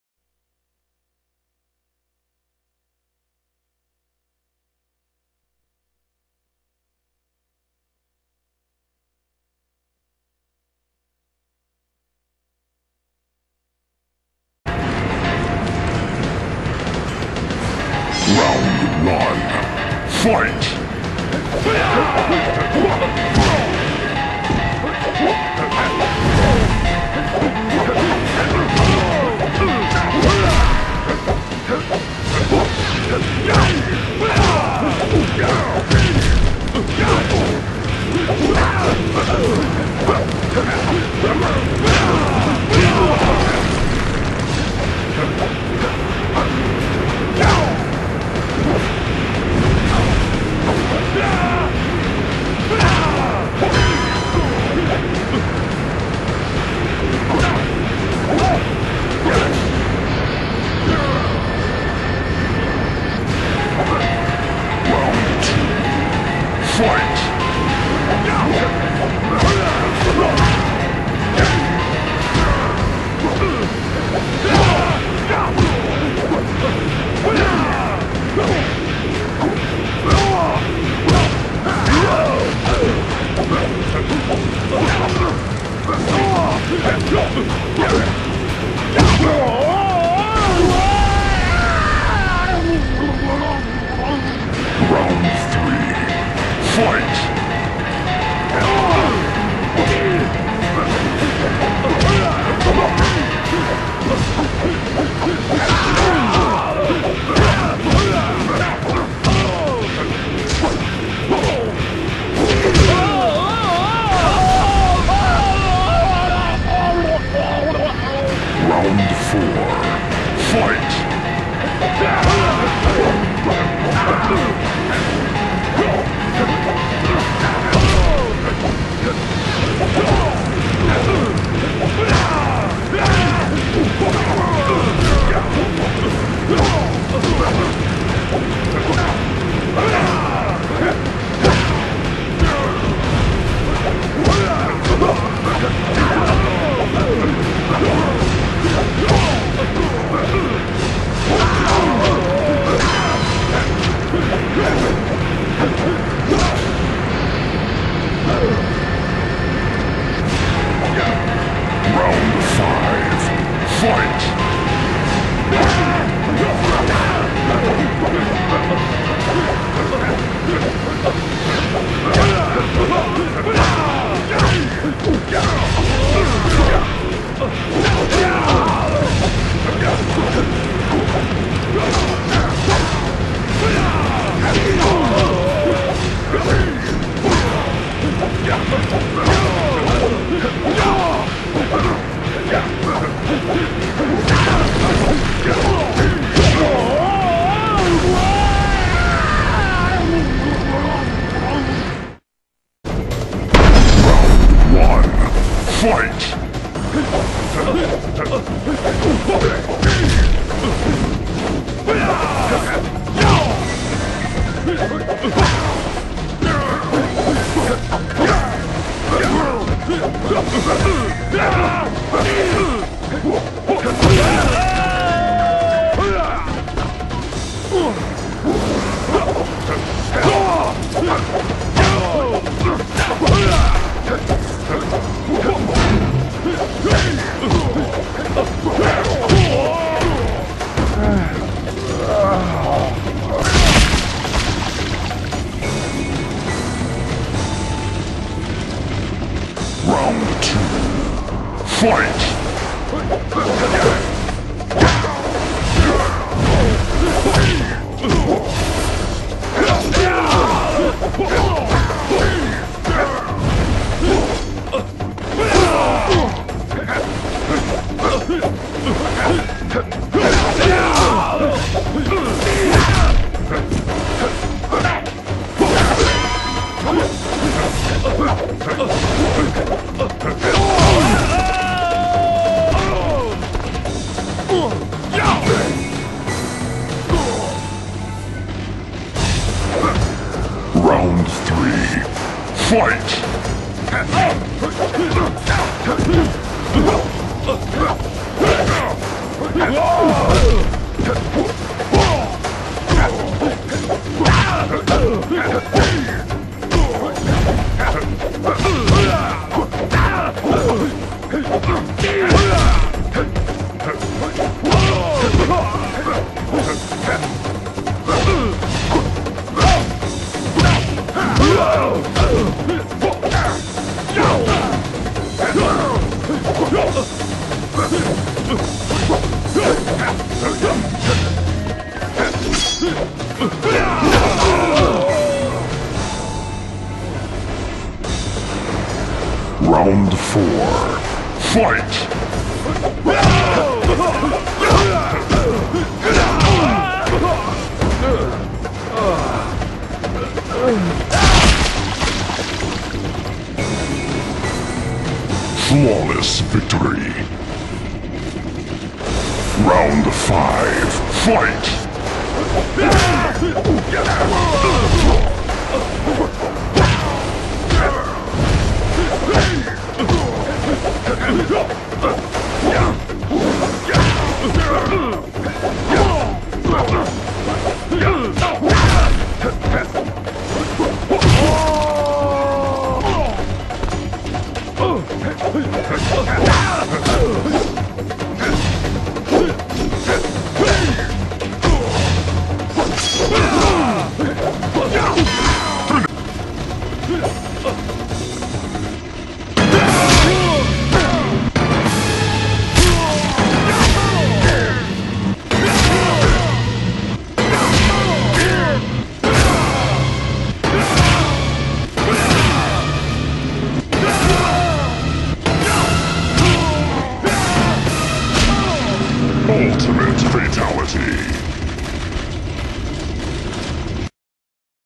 Battle Between Good & Evil: Dragonfire vs. DarkDragonfire Final Part
At last, the end to the greatest struggle in Dragonfire's life. One shall stand, one shall die!

And now, enjoy the video as this will conclude the battle between good and evil.